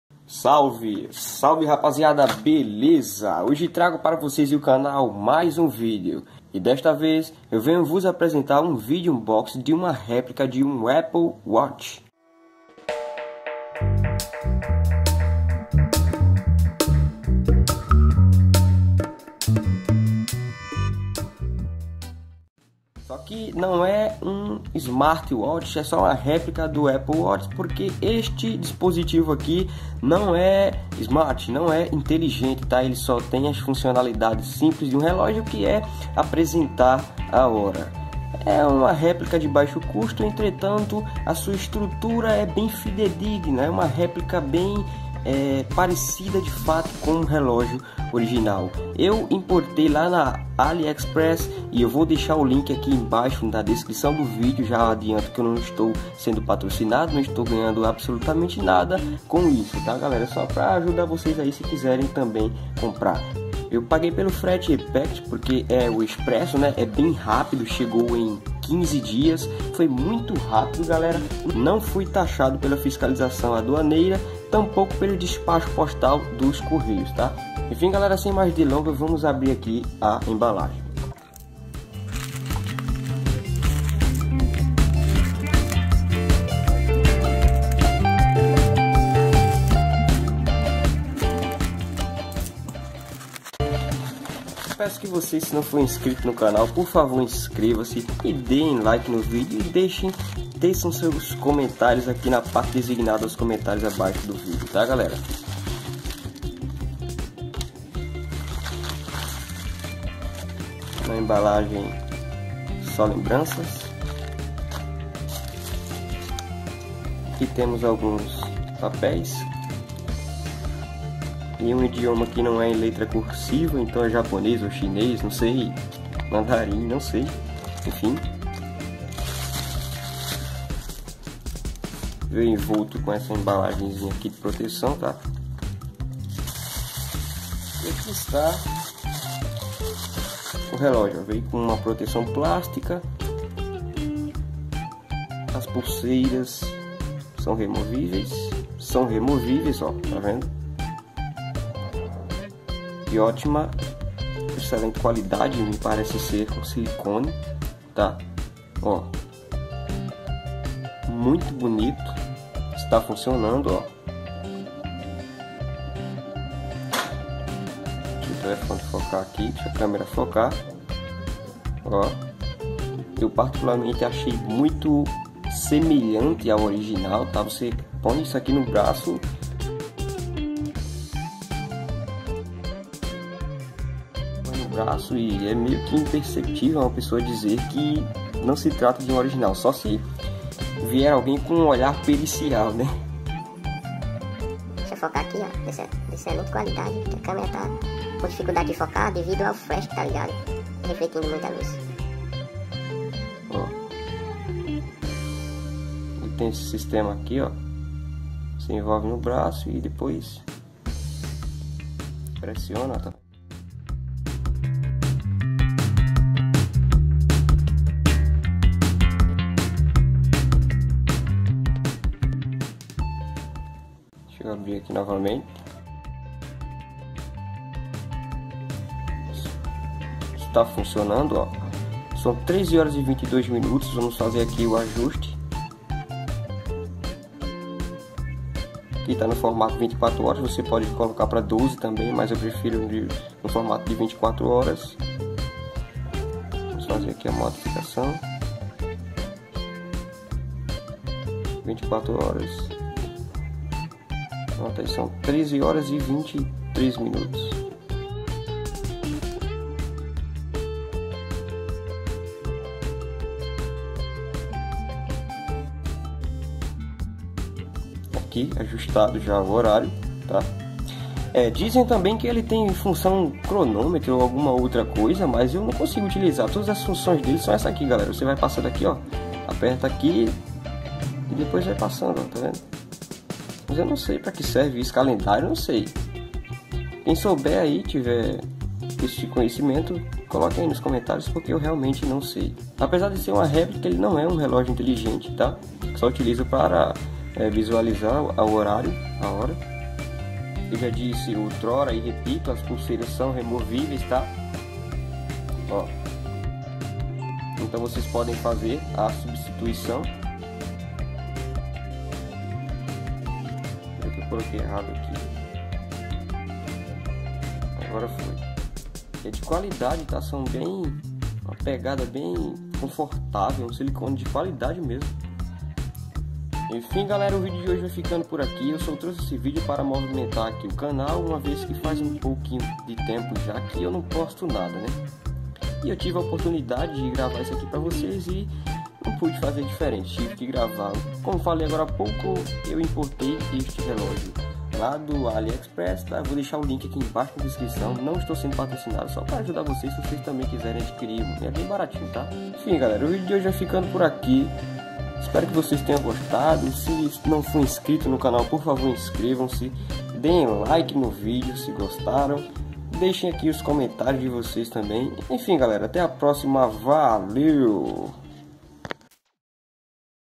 Unboxing Réplica do Apple Watch | HD | Português BRASIL
Unboxing Réplica de Baixo Custo do Apple Watch!
Unboxing Brasil.

NÃO É SMART
 **O relógio não tem a função touch, ele é simples, somente hora, regulável nos botões ao laterais.**

Você quer ter um relógio com design incrível e semelhante ao Apple Watch mas por um preço justo? Então acabou de encontrar o modelo que tanto procuras.

Se inscrevam no web canal e deem like para incentivar a produção de mais conteúdos relacionados!

Unboxing Brasil.